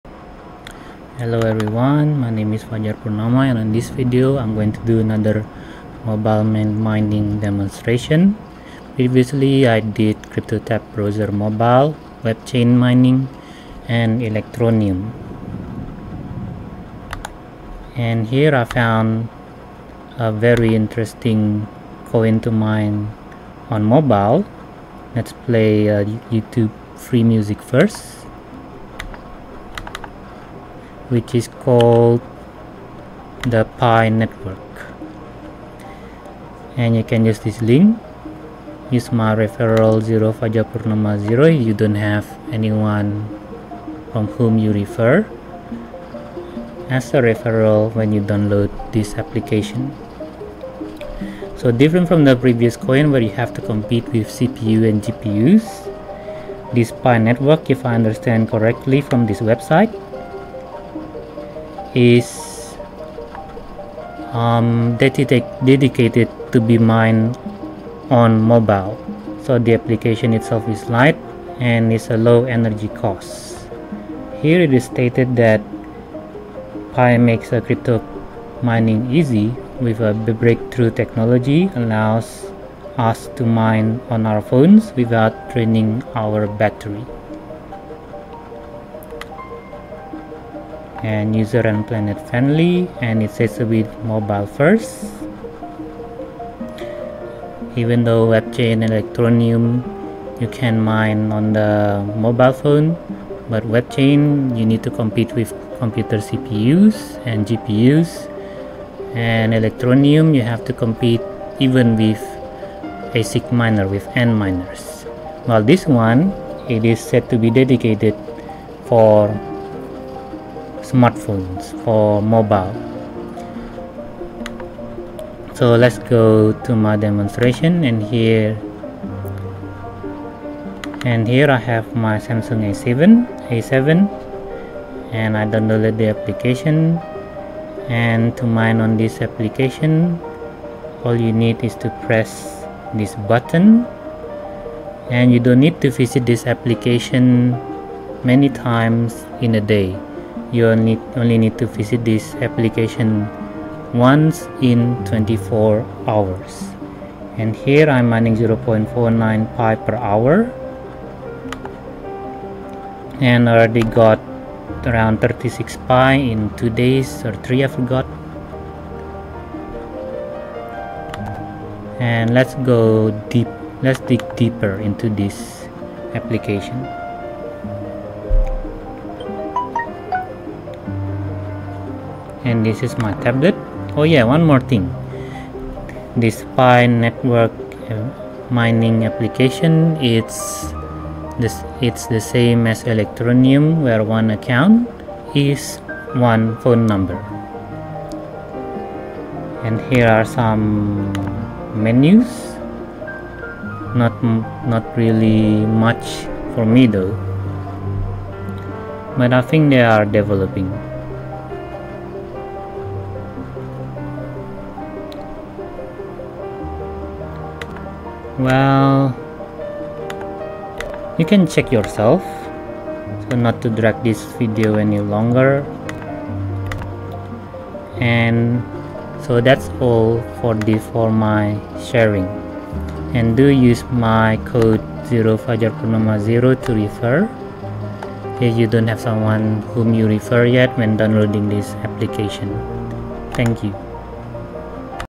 Cryptocurrency Mining Android Smartphone Pi Network
Different from the previous cryptocurrency videos that I mined on my mobile phone, is dedicated to be mined on mobile, and hopefully mobile only.